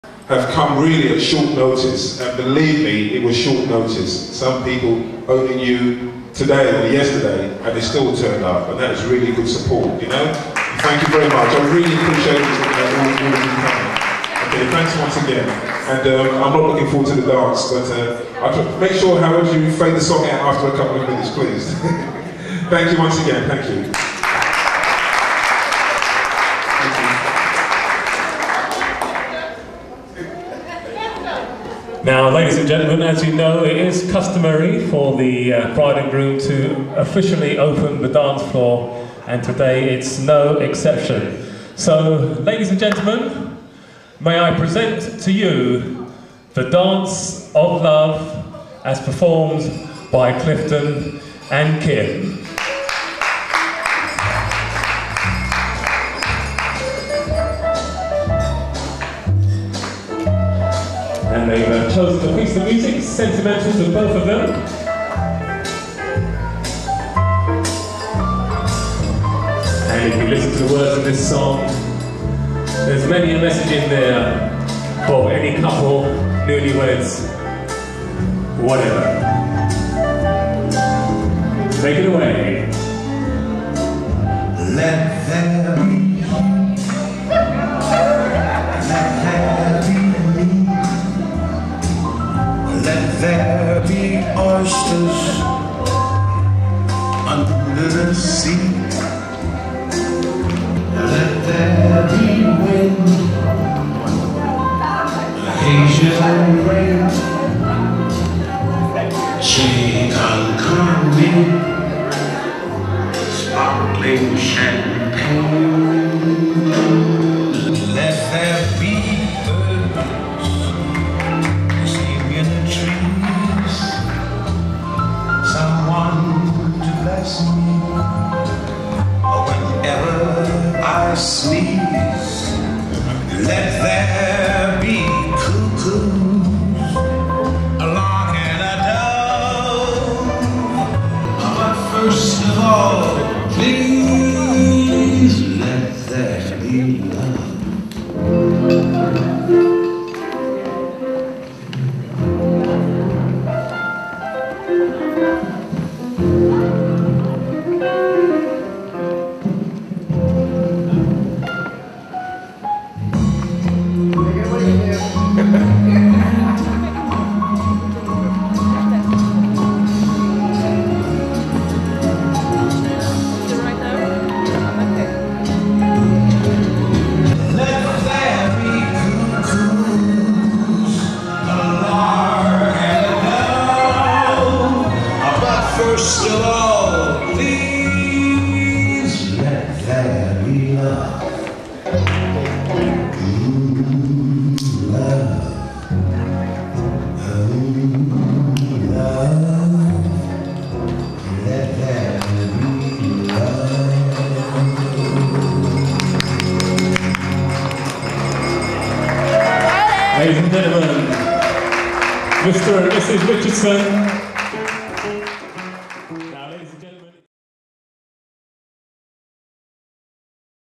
Kim and Cliff's Wedding 2003 Pt.5
My Wedding Day 2003 Pt. 5.

Our wedding day back in 2003. it feels like it was just the other day. What an absolutely lovely day we had...many friends and family attended this special occasion which was held at the Connaught Rooms in Central London. I had lost the original video recording, but was lucky to come across a business card belonging to the camera man who still had his original copy!